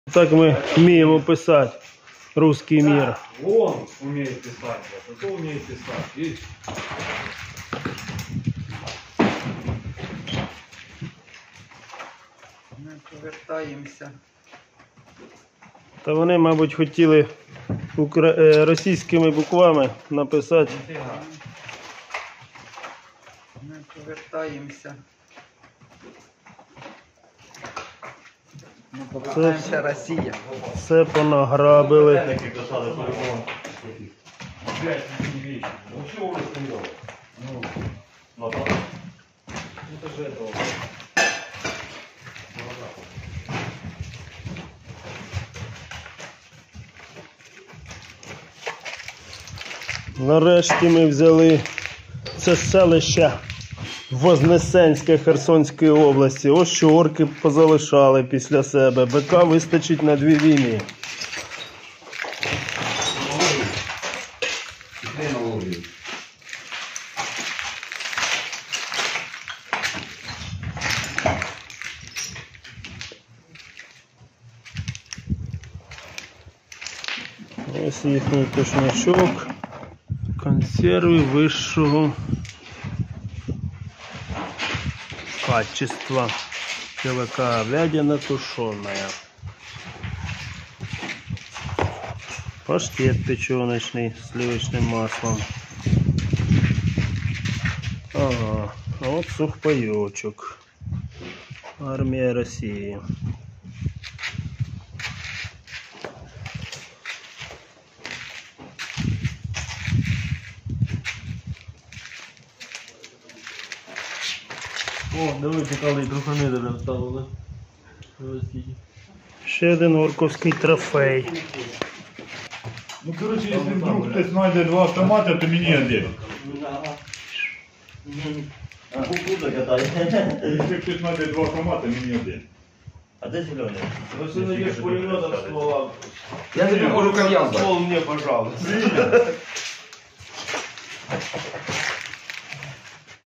Військові одного з підрозділів 60-ї ОПБр звільнили ще один населений пункт на Херсонщині
Журналісти “Наше місто” отримали ексклюзивні кадри з деокупованого селища.

Кілька днів велись запеклі бої з російськими окупантами.

Наші захисники не відступали ні на крок і в результаті змогли прогнати ворогів.

Українські військові були в чергове шоковані тим, що залишили після себе рашисти.

Розбиті домівки, гори сміття і хаос...

Схоже, що базувалися вони в одній з місцевих шкіл.

Тікали так швидко, що лишили після себе купу ящиків з боєкомплектами, провізію.

Також не встигли забрати з собою і все награбоване.

Герої дуже втомлені після кількох безсонних ночей, проте не втрачають оптимізму.

Кажуть, що все обов'язково відвоюємо, треба тільки трохи часу